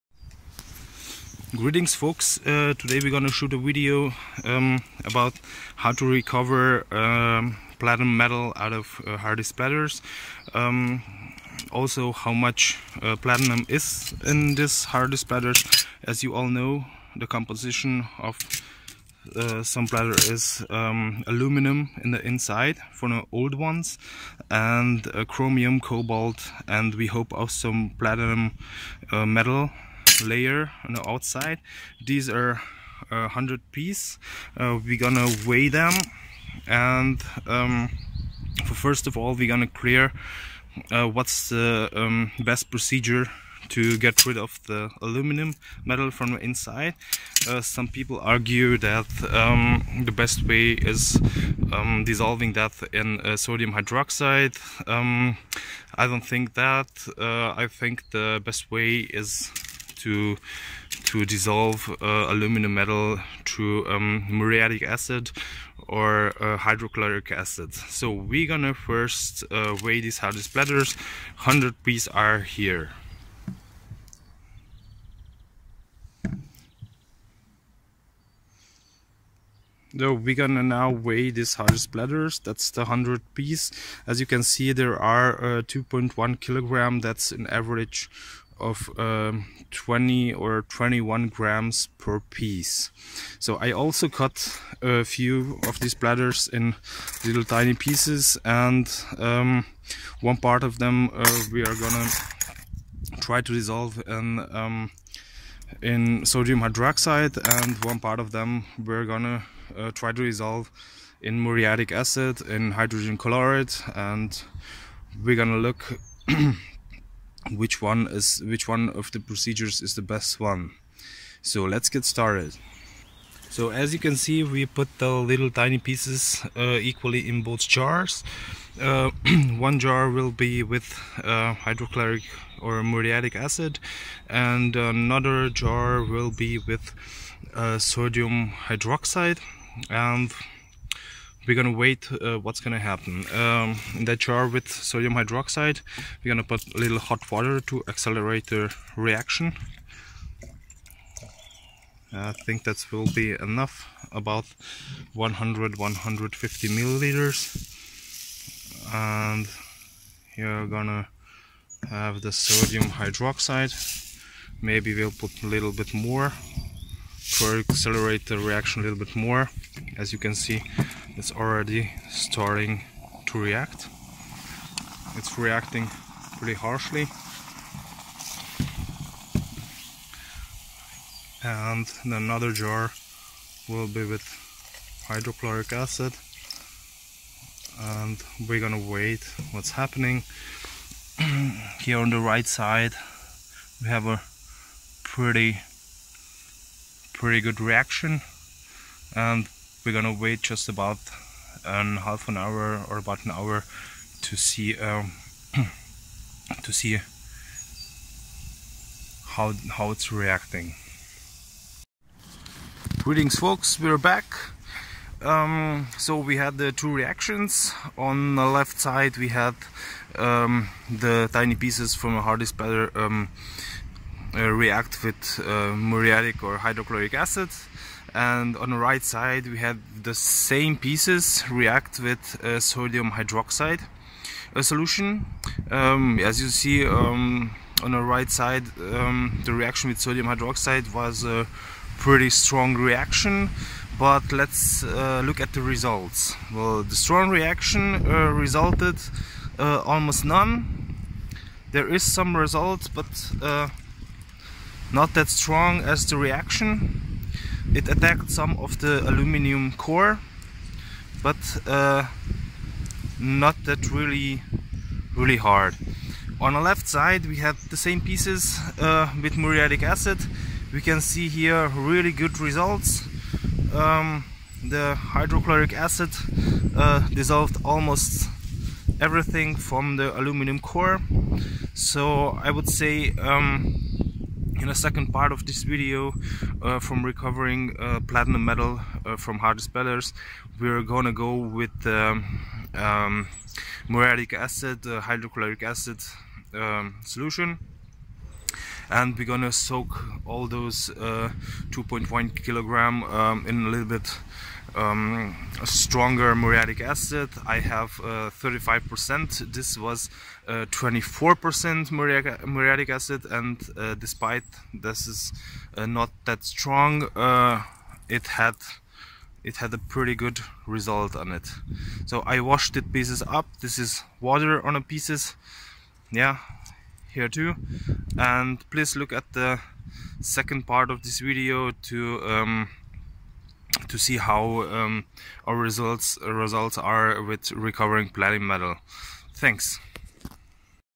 How much Platinum is actualy in Hard disk Platters Part 1
In this video we are going to show you 2 ways of how you start recovering Platinum Metal out of Hard disk Platters also coming to a conclusion of which one is the best for an optimal recovery.